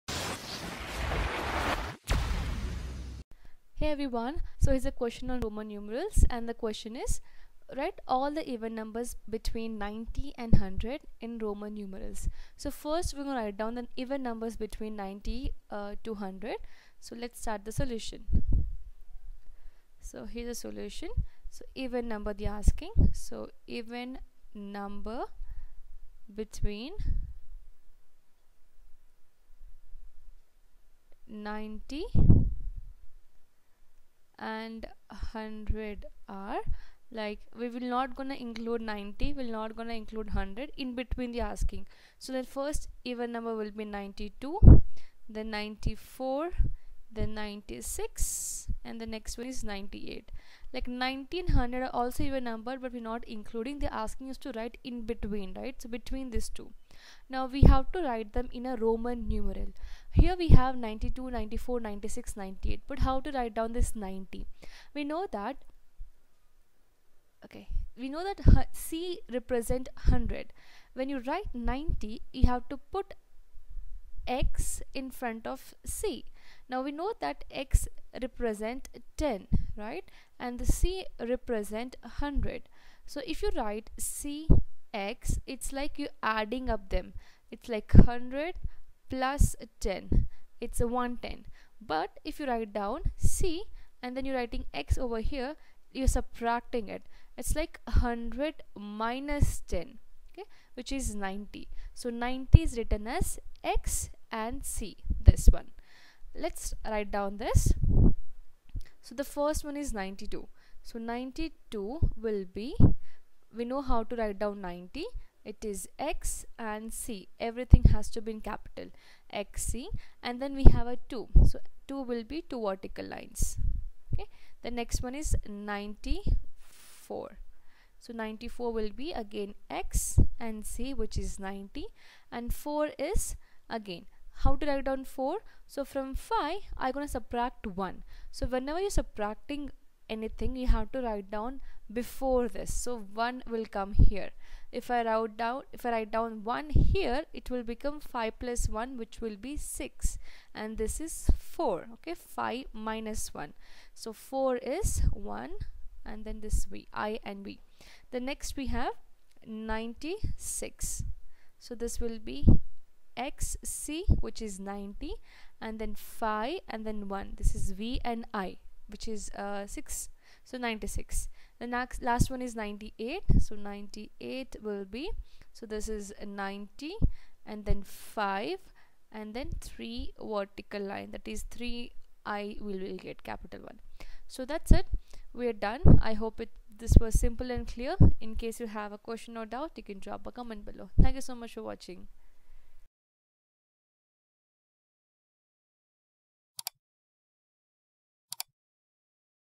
Write all the even numbers between 90 and 100 in Roman Numerals // Roman numerals
Write all the even numbers between 90 and 100 in Roman Numerals